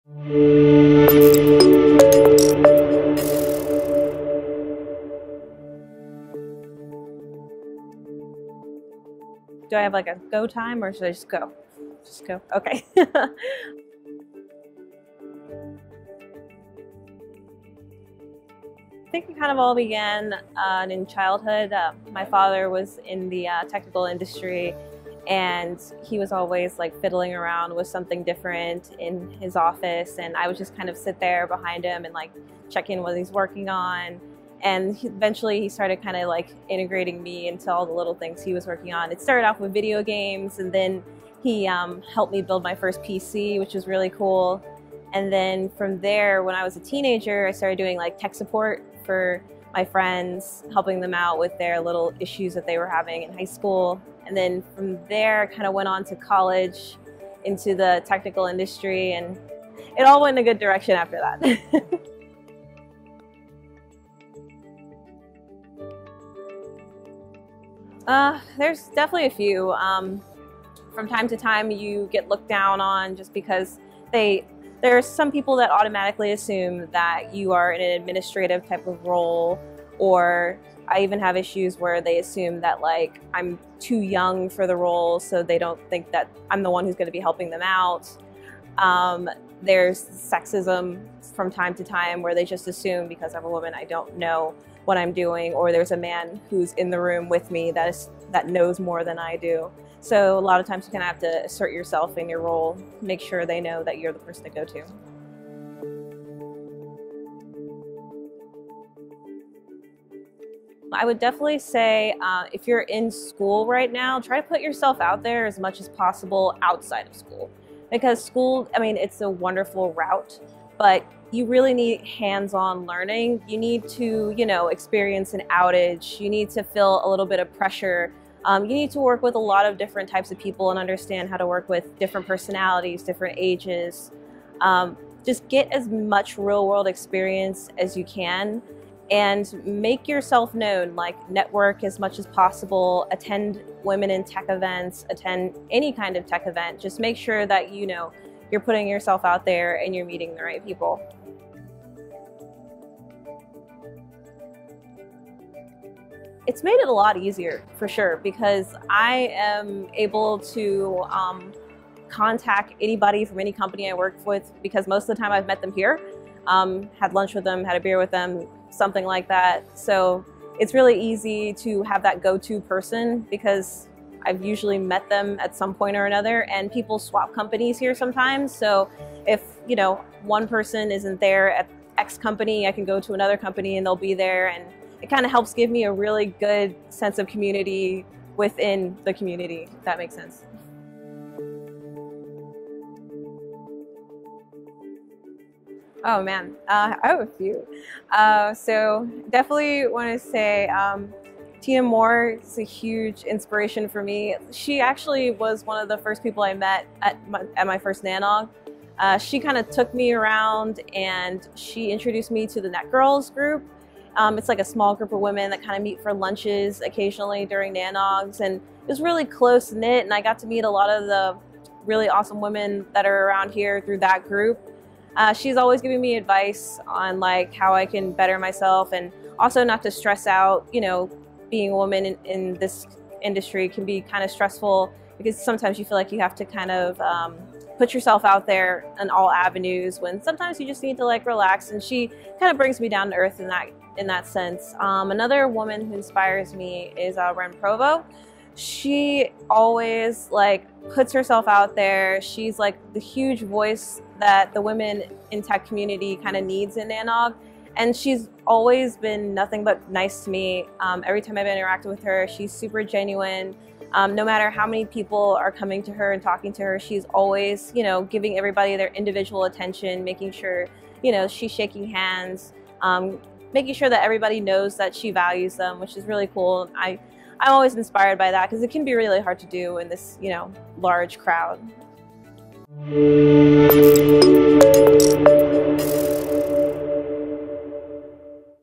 NANOG Women in Tech - A Conversation with Micheala Clifford
Our newest series on NANOG TV further explores the stories and career paths of some of the most exceptional women we know. Watch our first interview, featuring Micheala Clifford of Netflix, to learn more about her experience as a woman navigating the world of network engineering.